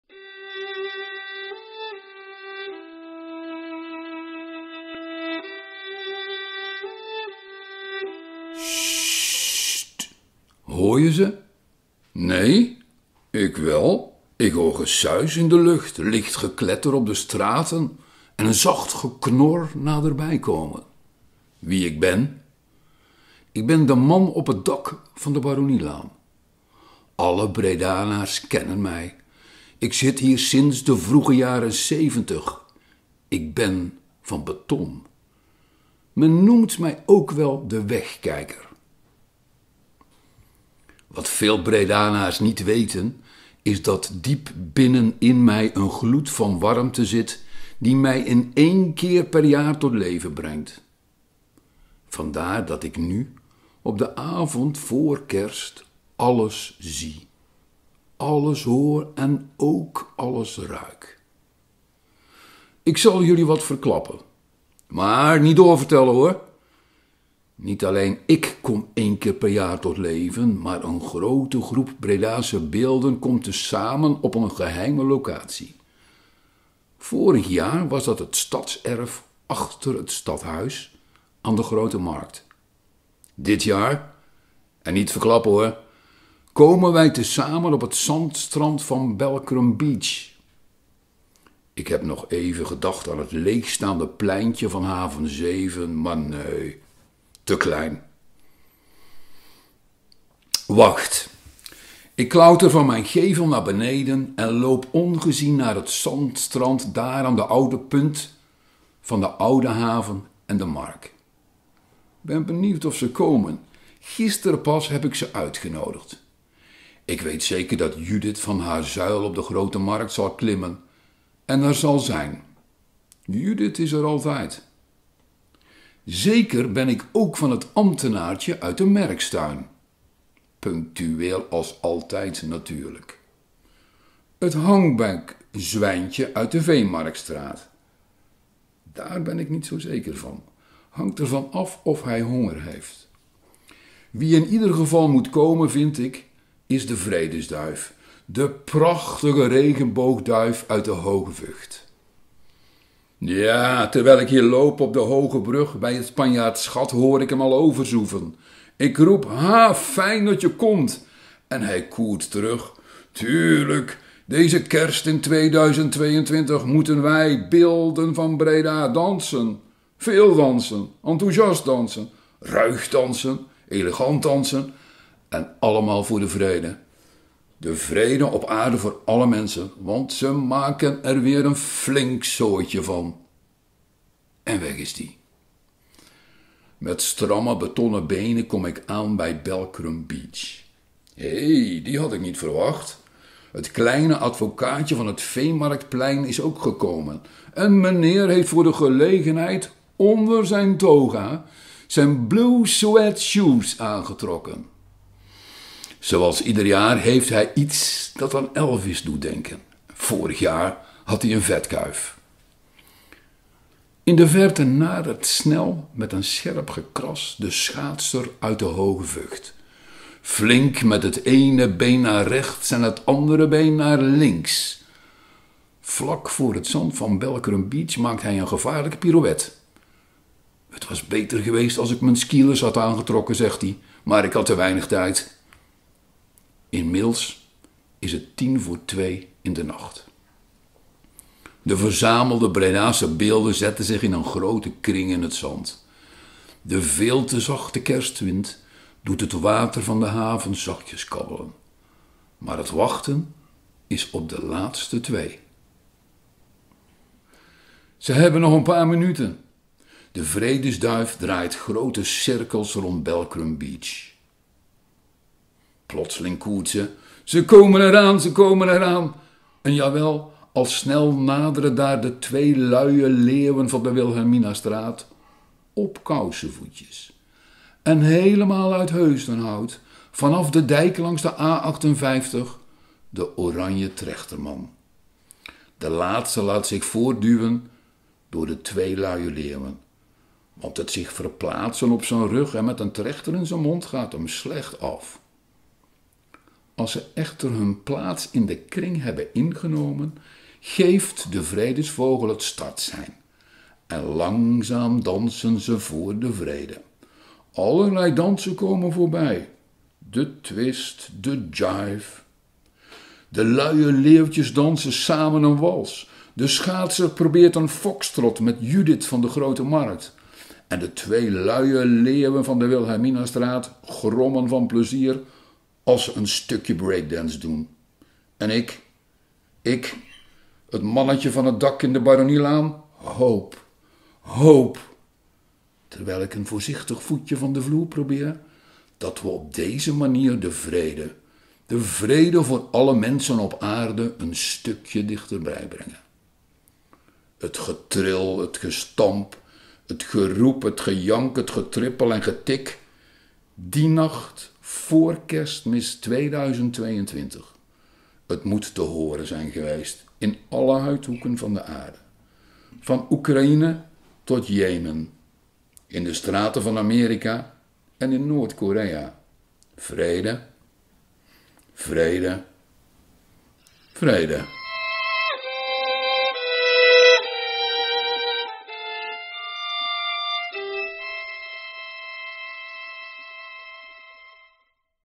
BM 2022 kerstverhaal Dansende beelden
Dit Kerstverhaal is voorgedragen als intermezzo van het Advents-/kerstconcert van de Koninklijke Zangvereniging Breda’s Mannenkoor op 11 december 2022 in de Sacramentskerk te Breda.
Het verhaal gaat over standbeelden in Breda die in de nacht  voor kerstmis 2022 in het geheim bij elkaar komen om er met veel kabaal te gaan dansen als oproep naar alle uithoeken van de aarde, een oproep voor vrede!.

Idee, tekst en voordracht:  Paul van Driessche
Fotografie en montage: Simon Keep